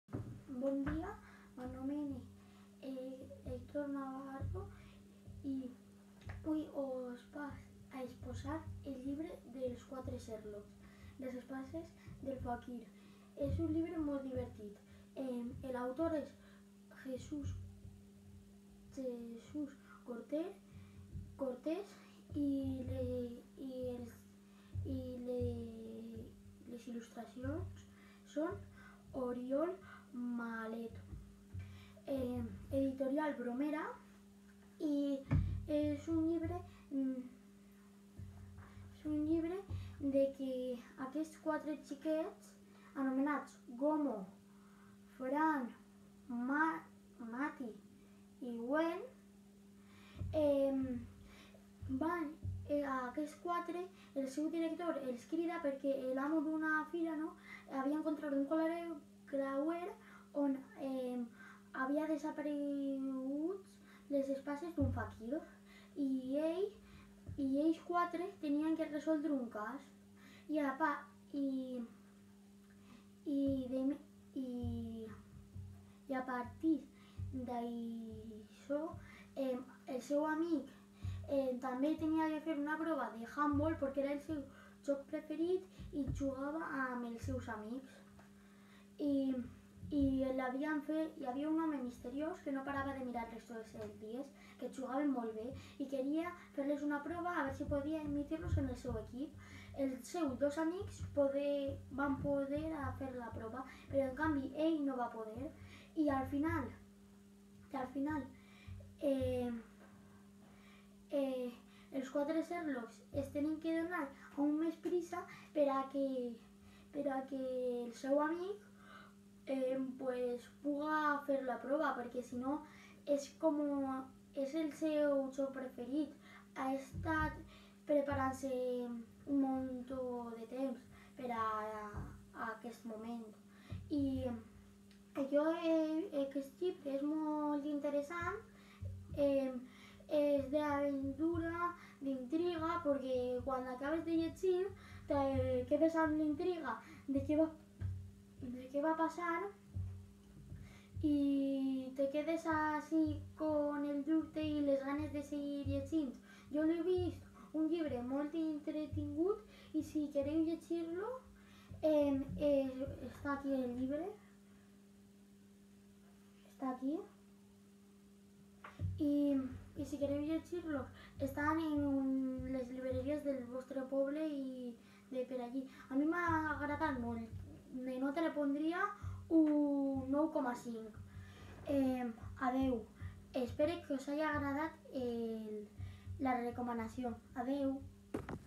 Hèctor Navarro recomana Les espases del faquir, de Jesús Cortés
Fran, Gomo, Mati i Wen hauran de posar en pràctica totes les seues habilitats detectivesques per a ajudar Blai, un bon amic jugador d’handbol acusat del robatori d’unes espases en una fira d’atraccions. La seua carrera esportiva corre perill i les proves del cas no deixen cap dubte: Blai n’és el culpable. On són les espases?
Un nou cas per a aquests quatre amics apassionats de l’esport i de les aventures de Sherlock Holmes.
Fes-te detectiu amb aquesta sèrie!